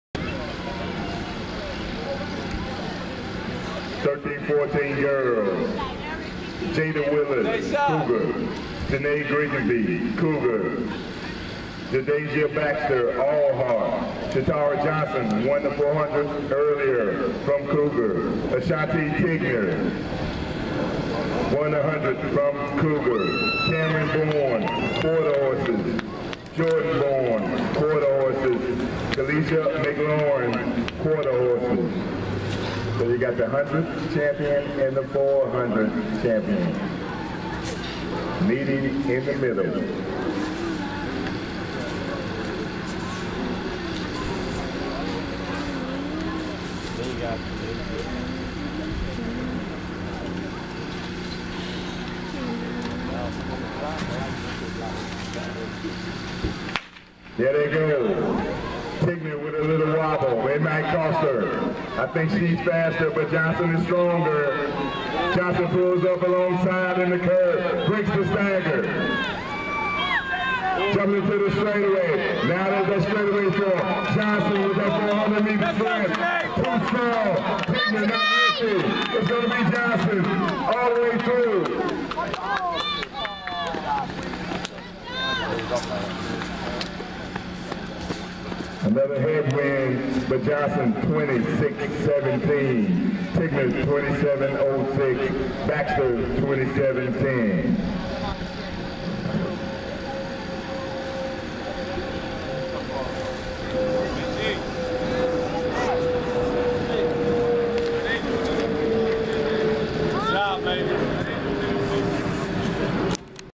Tenae` with Ashanti & ShaTara 200m Finals June 7, 2015 All Heart Relays
We're so proud of Tenae` for getting out there and putting in work.. Her first week back after her injury and she's working hard... Great job girls!!